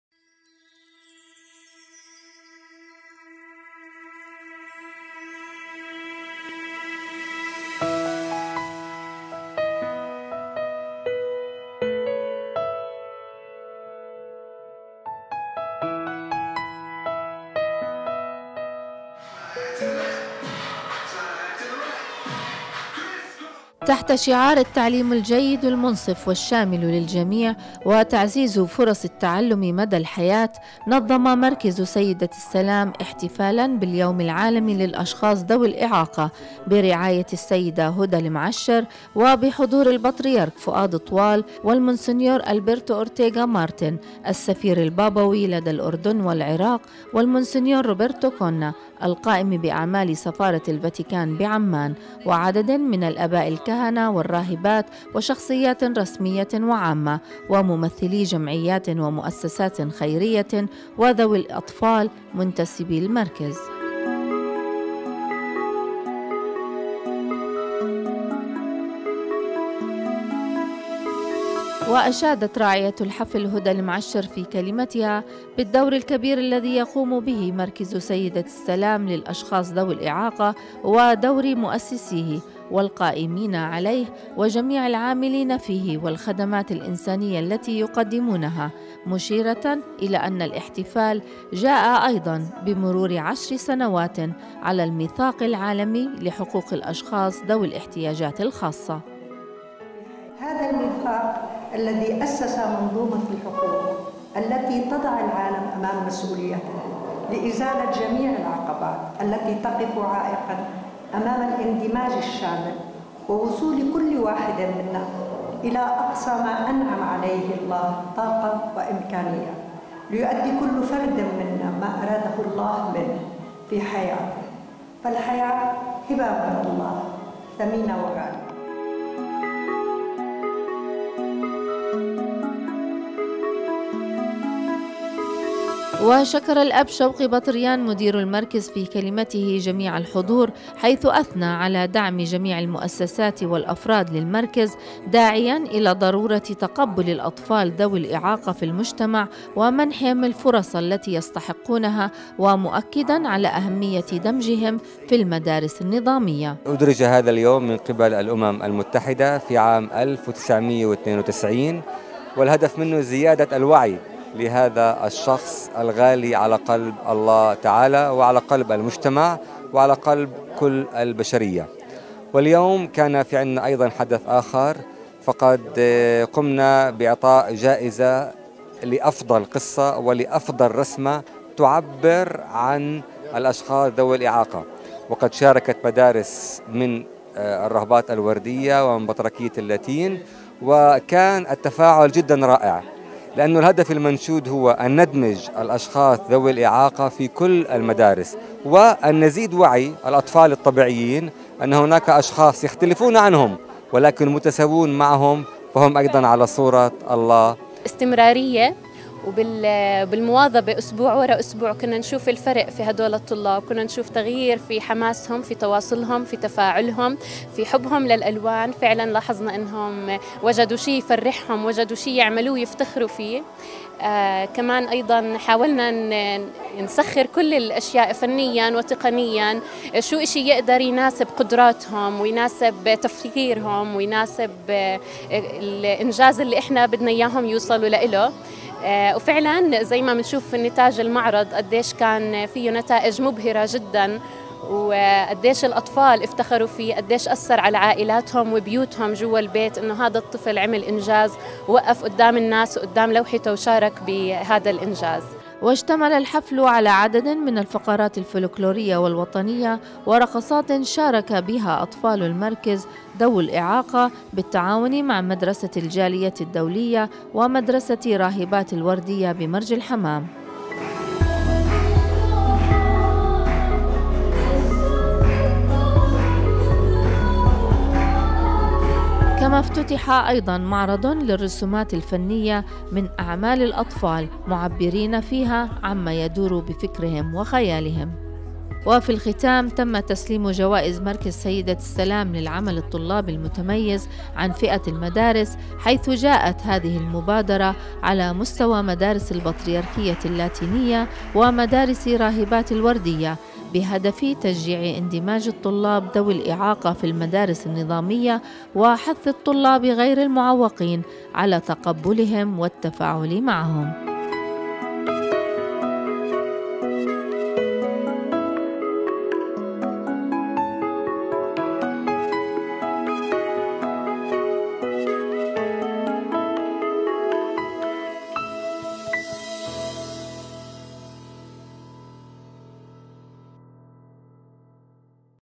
نورسات الأردن : تقرير - مركزُ سيدةِ السلام يحتفلُ باليومِ العالمي للأشخاصِ ذوي الاعاقة 2016
تحت شعار "التعليم الجيد المنصف والشامل للجميع وتعزيز فرص التعلم مدى الحياة " نظم مركز سيدة السلام احتفالا باليوم العالمي للأشخاص ذوي الإعاقة ،برعاية السيدة هدى المعشر وبحضور البطريرك فؤاد طوال والمطران سليم الصايغ والمونسنيور البيرتو اورتيغا مارتن السفير البابوي لدى الأردن والعراق والمونسنيور روبيرتو كونا القائم باعمال سفارة الفاتيكان بعمان وعدد من الآباء الكهنة والراهبات وشخصيات رسمية وعامة وممثلي جمعيات ومؤسسات خيرية وذوي الأطفال منتسبي المركز.
واشادت راعية الحفل هدى المعشر في كلمتها بالدور الكبير الذي يقوم به مركز سيدة السلام للأشخاص ذوي الإعاقة ودور مؤسسيه والقائمين عليه وجميع العاملين فيه والخدمات الإنسانية التي يقدمونها مشيرة أن الاحتفال جاء ايضا بمرور عشر سنوات على الميثاق العالمي لحقوق الأشخاص ذوي الاحتياجات الخاصة. هذا الميثاق الذي وضع العالم أمام مسؤوليته لإزالة جميع العقبات التي تقف عائقًا أمام الإندماج الشامل ووصول كل واحد منا إلى أقصى ما أنعم عليه الله من طاقات وإمكانيات...
وشكر الأب شوقي بطريان مدير المركز في كلمته جميع الحضور حيث اثنى على دعم جميع المؤسسات والأفراد للمركز ..داعياً إلى ضرورة تقبل الأطفال ذوي الإعاقة في المجتمع ومنحهم الفرص التي يستحقونها ومؤكداً على أهمية دمجهم في المدارس النظامية..
واشتمل الحفل على العديد من الفقرات الفلكلورية والوطنية ورقصات شارك بها اطفال المركز ذوي الإعاقة بالتعاون مع مدرسة الجالية الدولية ومدرسة راهبات الوردية بمرج الحمام... كما افتتح ايضا معرضا للرسومات الفنية من اعمال الأطفال معبرين فيها عما يدور بفكرهم وخيالهم.
وفي الختام تمَّ تسليم جوائز مركز سيدة السلام للعمل الطلابي المتميز عن فئة المدارس حيث جاءت هذه المبادرة بداية على مستوى مدارس البطريركية اللاتينية في الأردن ومدارس راهبات الوردية بهدف تشجيع اندماج الطلاب ذوي الإعاقة في المدارس التنظيمية، وحثِّ الطلاب غير المعوقين على تقبلهم والتفاعل معهم.